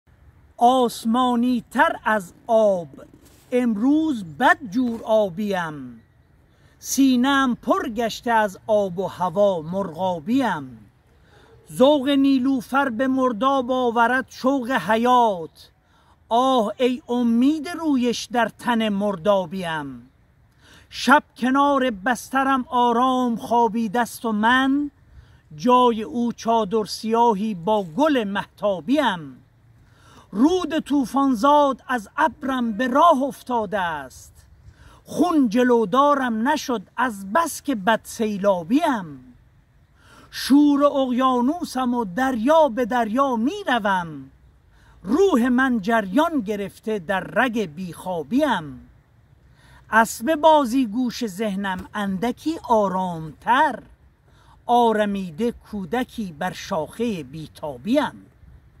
آسمانی تر از آب، امروز بدجور آبی ام سینه ام... akbarzadehbaghban@
آسمانی تر از آب ، امروز بدجور آبی ام
سینه ام پُر گشته از آب و هوا ، مرغابی ام

ذوق نیلوفر به مرداب آورد شوق حیات
آه ای امید رویش در تن مردابی ام

 شب کنار بسترم آرام خوابیده است و من
جای او چادر سیاهی با گل مهتابی ام

 رود طوفان زاد از ابرم به راه افتاده است
خون جلودارم نشد از بس که بد سیلابی ام

شور اقیانوسم و دریا به دریا میروم
 روح من جریان گرفته در رگ بی خوابی ام

اسب بازیگوش ذهنم! اندکی آرام تر
 آرمیده کودکی بر شاخه ی بی تابی ام

غلامرضا اکبرزاده باغبان

هنرمندان دکلماتور :
کوروش حسینی
نورا یکتامقدم
محمد سعید محری

با تشکر از دوست و همکار گرامی ام جناب آقای مهران حقیقی فر که برای تهیه این کلیپ شعرخوانی بنده را همراهی کردند.